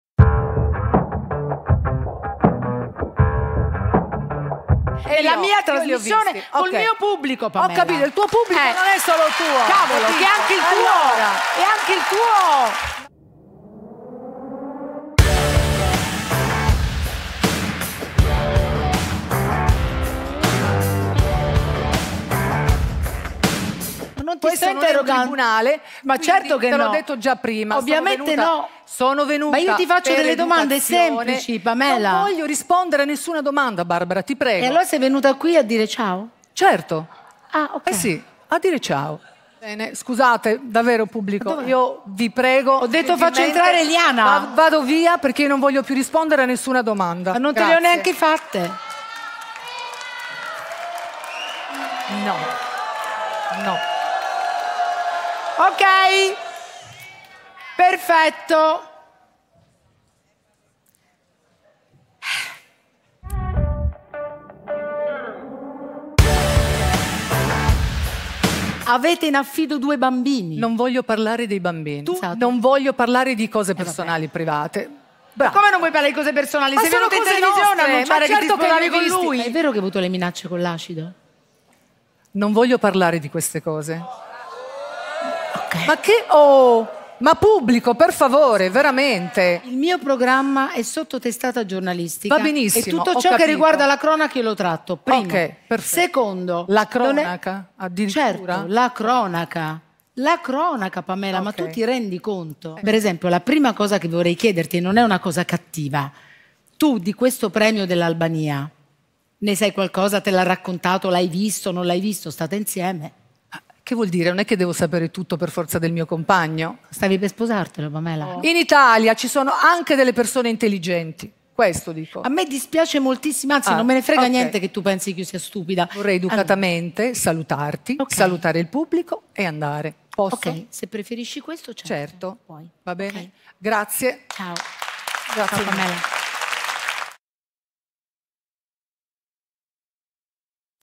Lo scontro tra Pamela Prati e Barbara d’Urso
I momenti migliori del botta e risposta tra Pamela Prati e Barbara d'Urso.